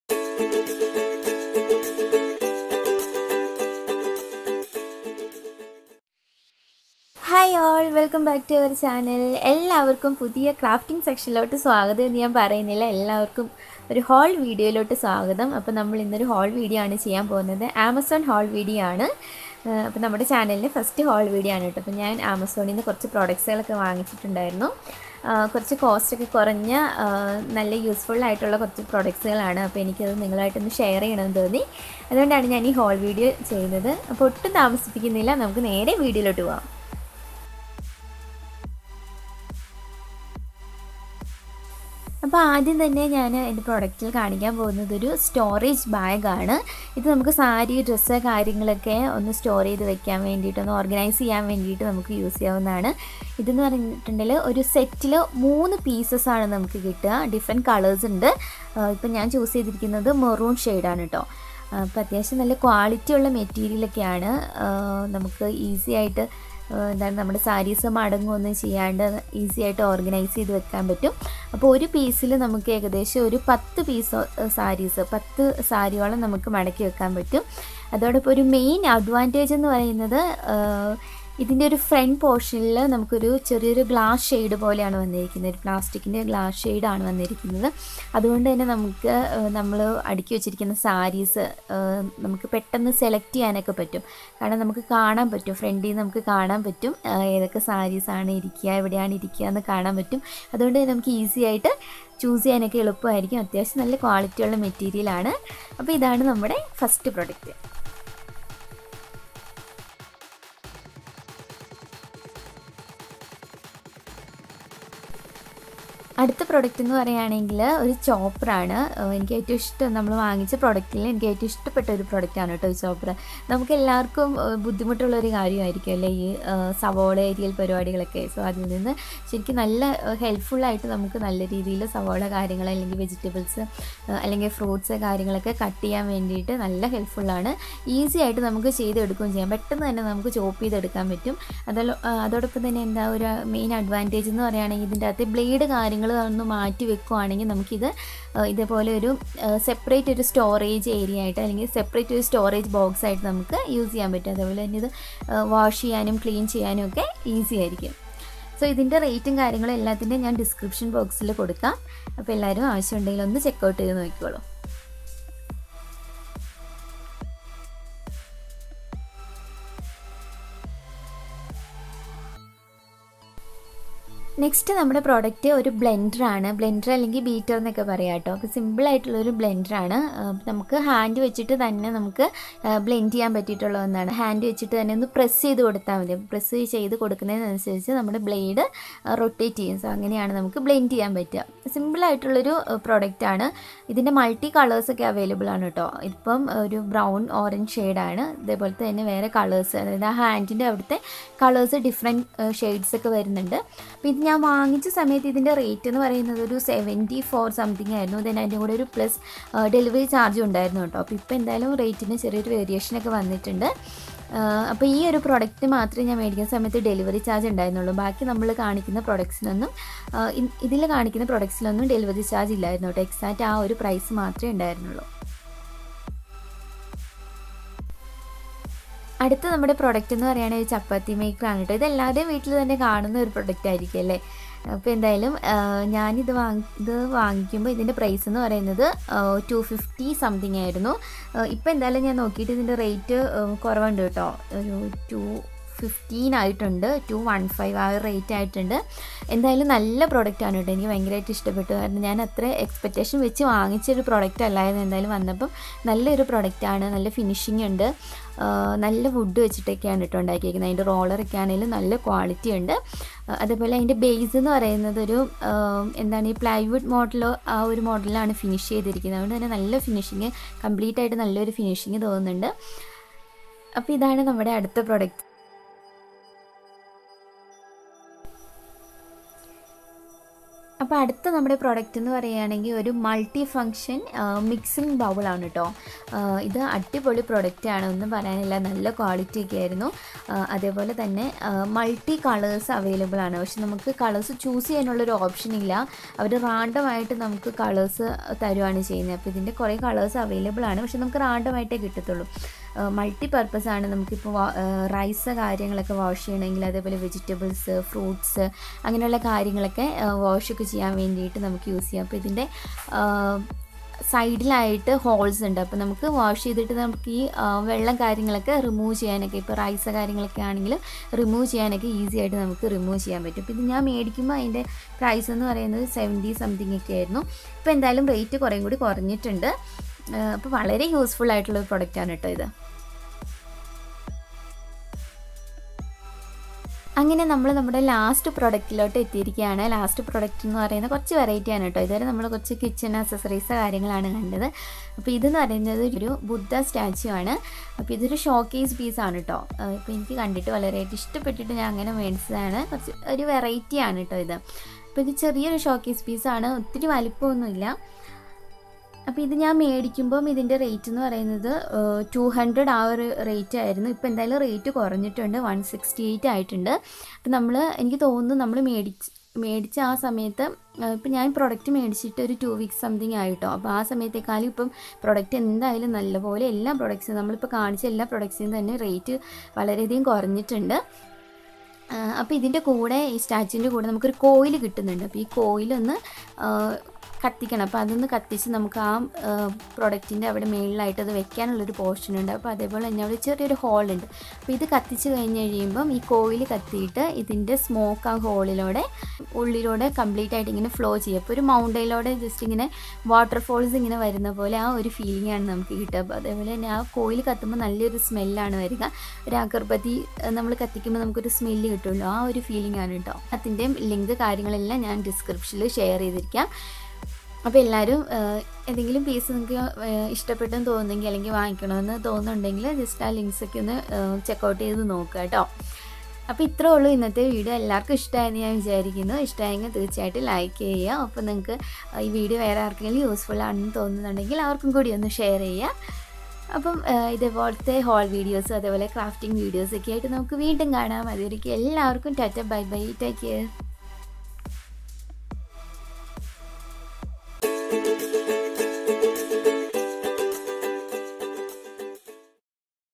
AMAZON HAUL/900 രൂപക്ക് താഴെ അടിപൊളി സാധനങ്ങൾ / AMAZON SHOPPING HAUL MALAYALAM
Amazon Shopping Links:

Non Woven Saree Cover Set

Wooden Chapati Maker

Mixing BowlMixing Bowl

Hand Blender and Beater

Manual Food Chopper

Buddha Smoke Fountain

Thank You Friends 😘😘 LOVE YOU ALL🥰😊